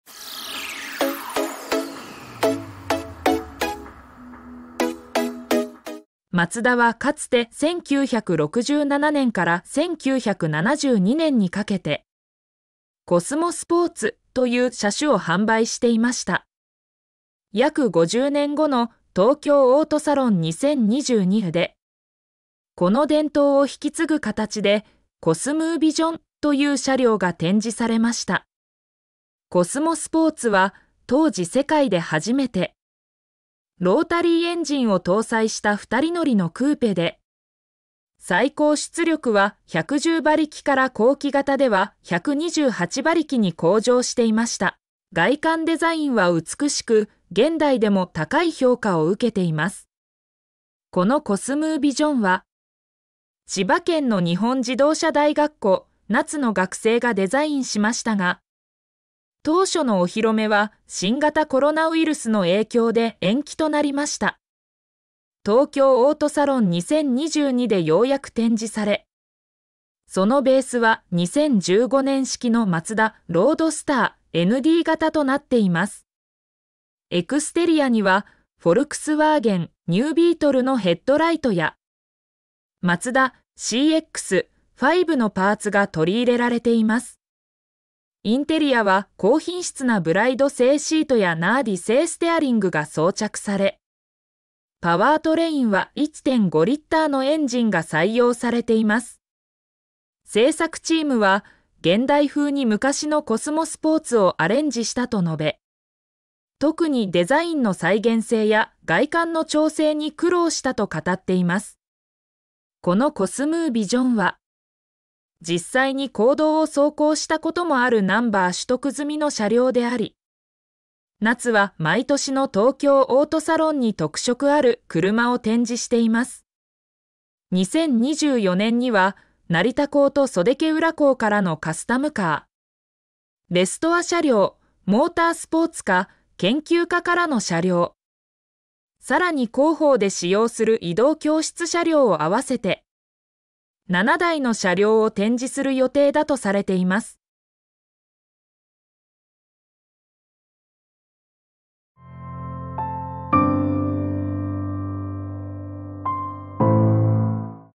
マツダ「コスモスポーツ」50年ぶり復活！「COSMO VISION」のワイドな特徴は？ | 車の話
コスモスポーツは、1967年に世界で初めてロータリーエンジンを搭載したモデルとして発売された2シータークーペです。